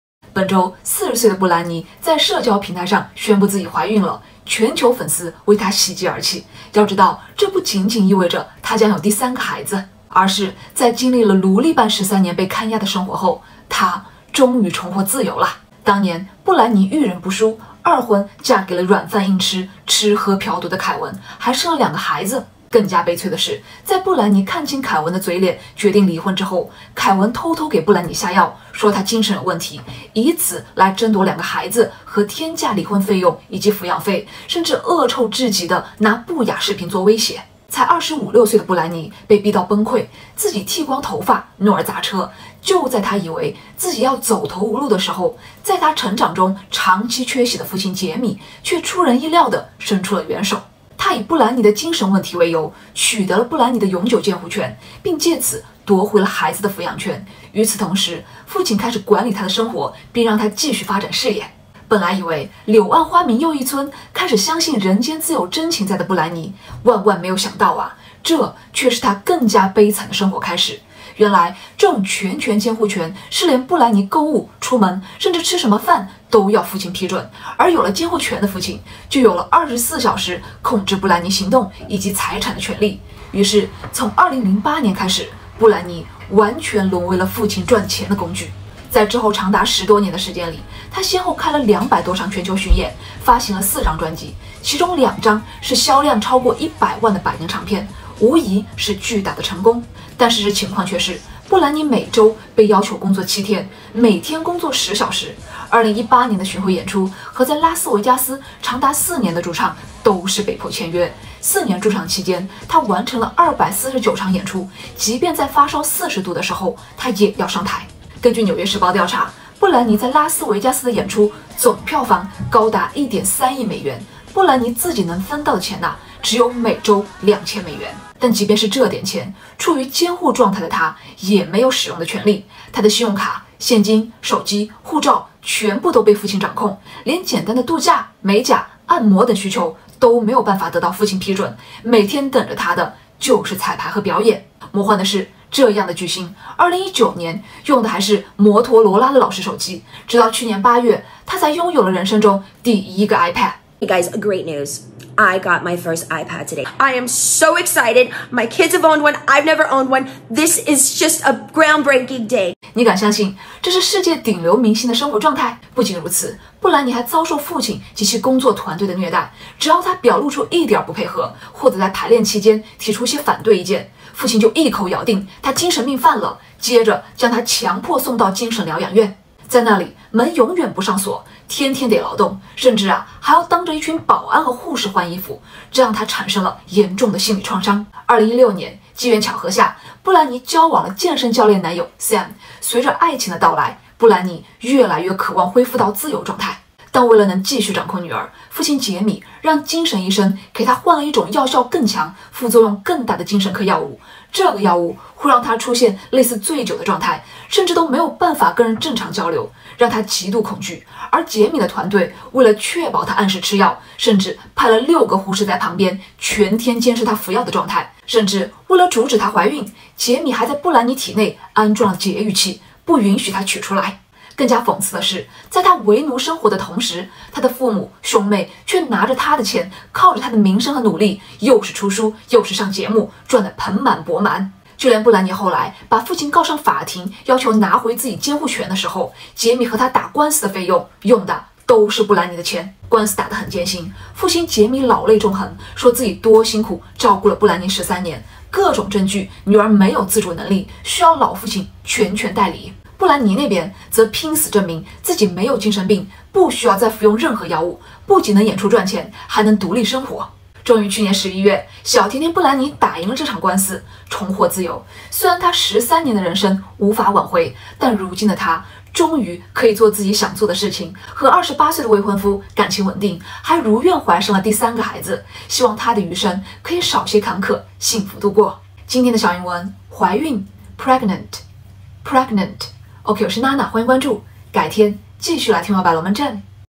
小甜甜布兰妮宣布怀孕，“为奴”13年终于获自由身
Nana的龙门阵Youtube唯一频道